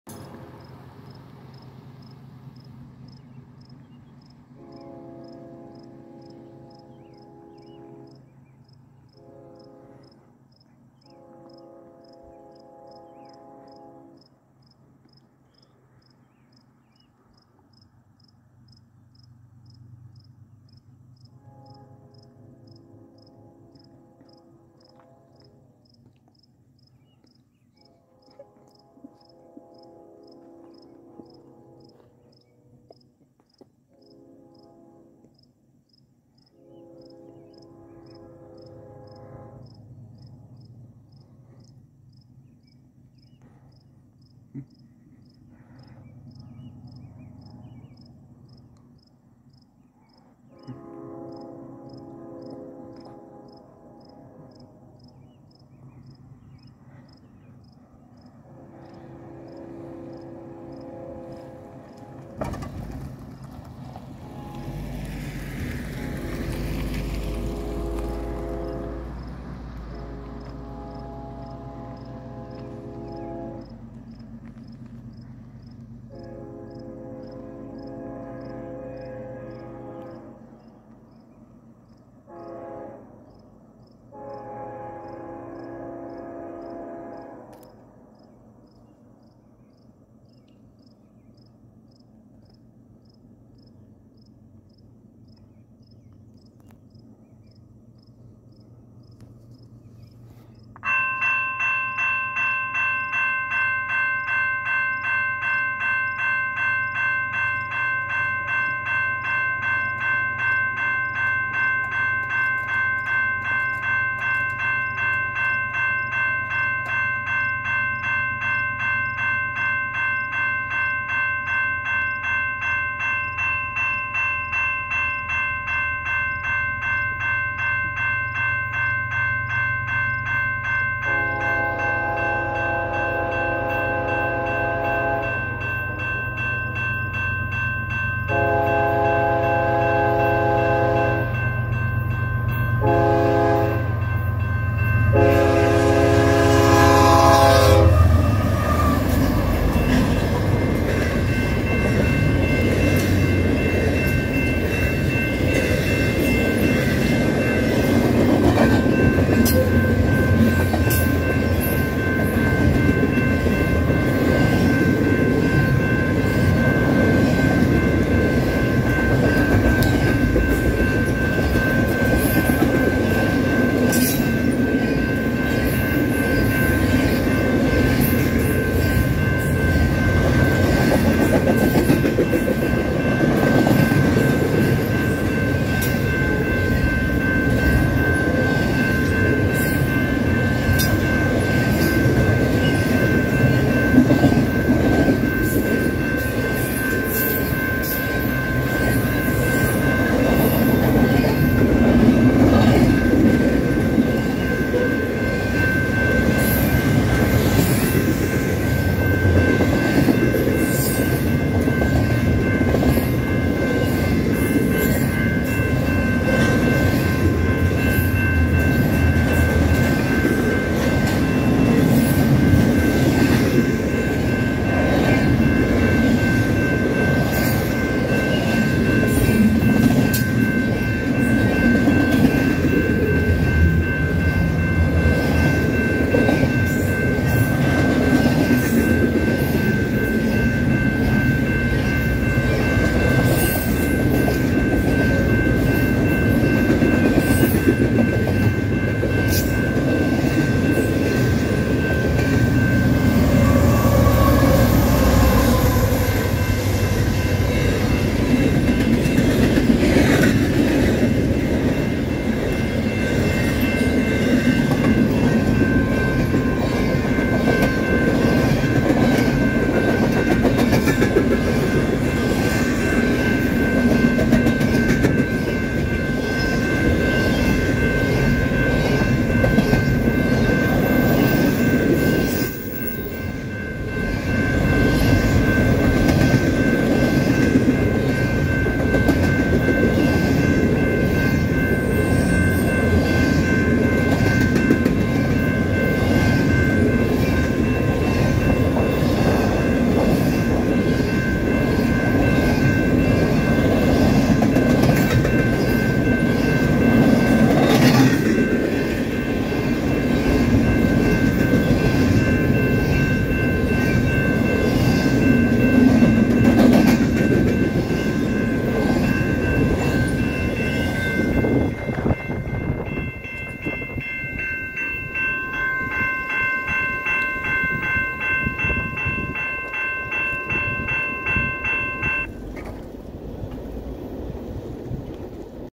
7/16/23 Great NF 2PC K5HL! ACU & KCSM DPU! Railroad Crossing, Mckenzie Road #1, Chelmsford ON
Notice the Lights Flashing in a Super Slow Speed. This Crossing is on the CPKC Cartier Sub.

Northern signal
Lights : Four 12"x20" Harmon Design GE LEDs inside WCH Flatback Frames
Bell : GS Type 2 E-Bell
Base : Siemens single sided
Light Bracket : Safetran
Gate Mechanism : Siemens Imprinted
Gate Lights : 4" RECO LEDs

Southern signal
Bell : None
Base : Siemems single sided
Light Bracket : Safetran

CPKC 113 WEST - CP 8727 - ES44AC - CP System Red w/o Beaver - Narrow Font K5HL w/ 2 Piece 1L
CP 7056 - SD70ACU - CP System Red w/ Beaver
DPU KCSM 4077 - SD70ACe - KCSM Southern Belle

Full 5:35 Video

Subdivision - CPKC Cartier Sub

Leave comments to tell me what you think about the video!

If you want to use my videos or clips, make sure to give my credit!